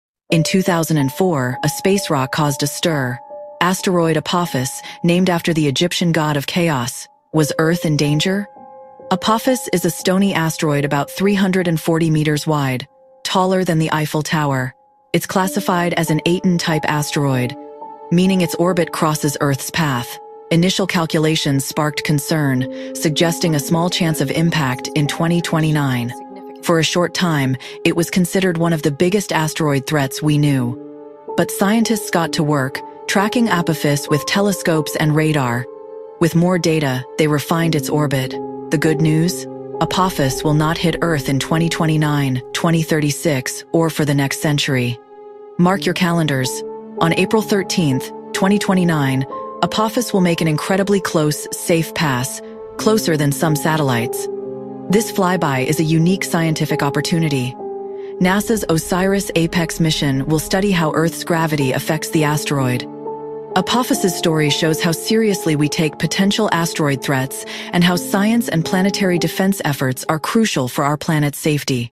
Asteroid impact in 2029???
Asteroid apophis could possibly hit Earth in 2029. Should we panic?